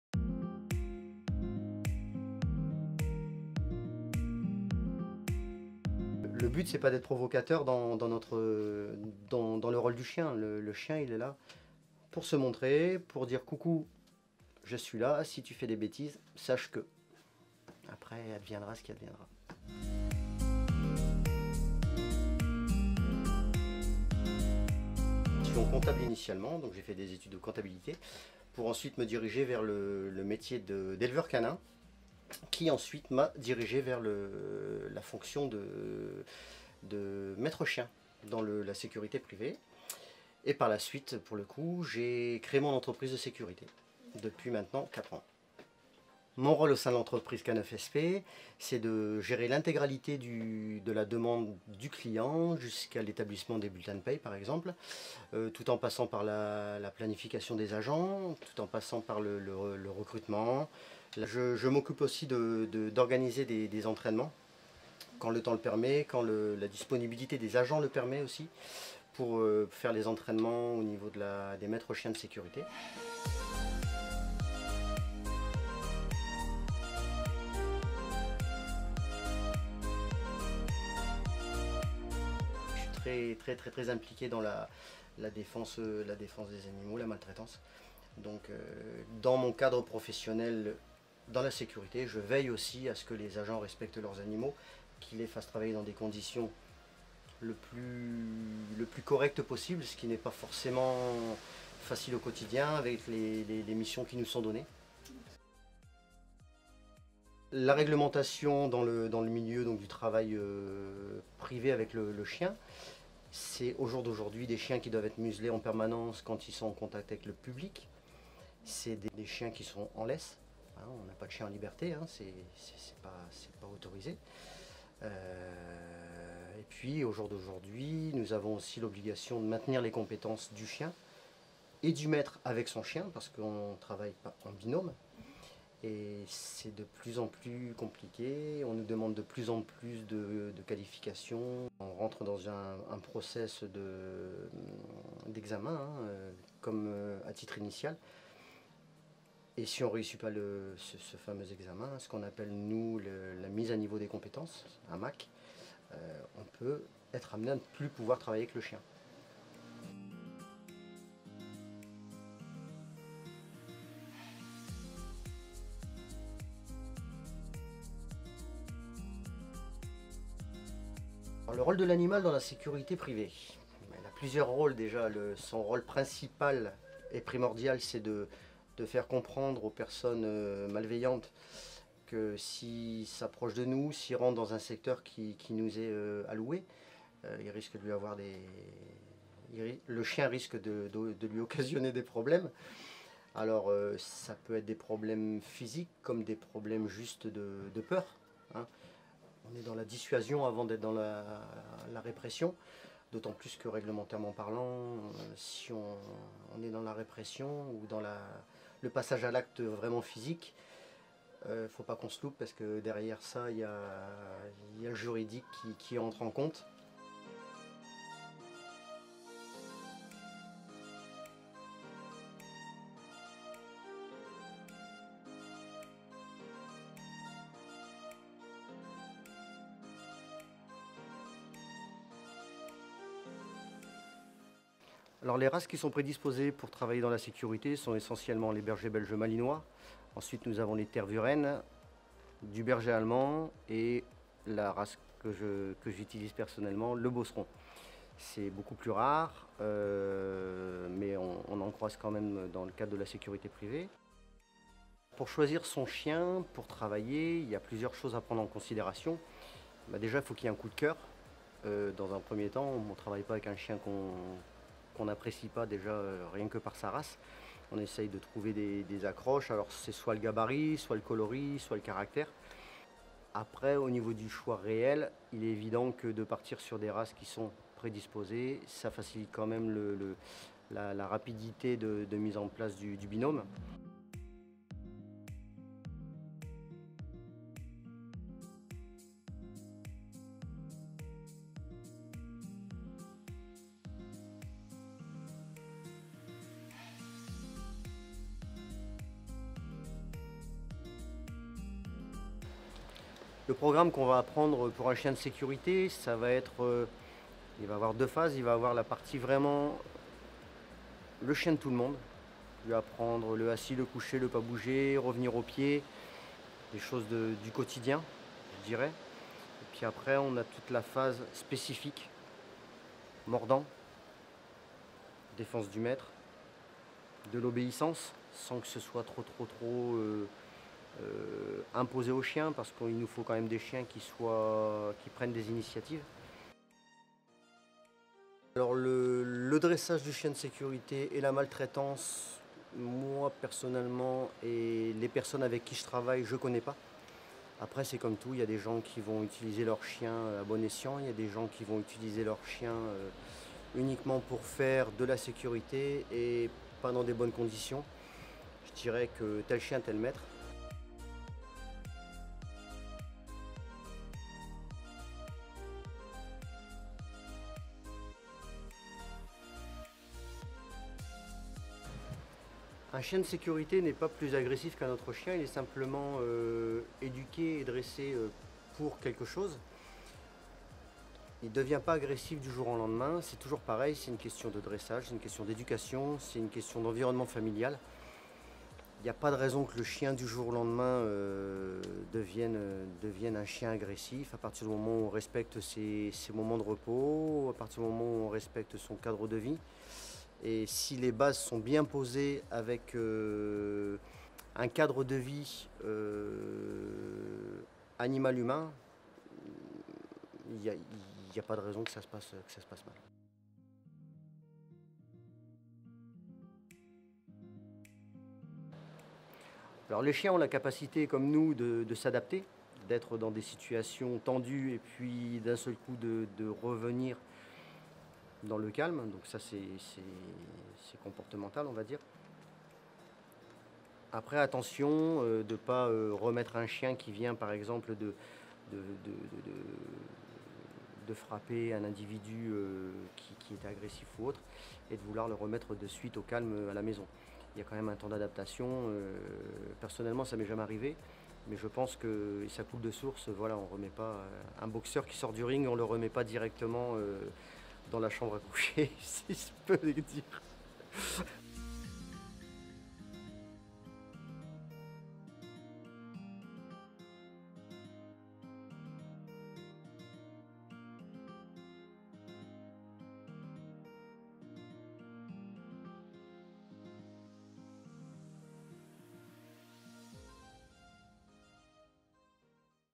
K9SP et l'un des métiers de la sécurité privée. Agent cynophile / Maître-chien / Conducteur canin
Être ou devenir maître-chien c'est avant tout avoir la passion du chien.

En quelques minutes, ce film vous parles de tous les aspects reliant le métier à la passion.

K9SP emploie régulièrement des maître-chien de sécurité privée pour répondre à la demande des clients dans le domaine du gardiennage de chantier, de sécurisation d'événement ou de site industriel.

en région PACA, basée à Avignon sur un secteur d'une heure aux alentours.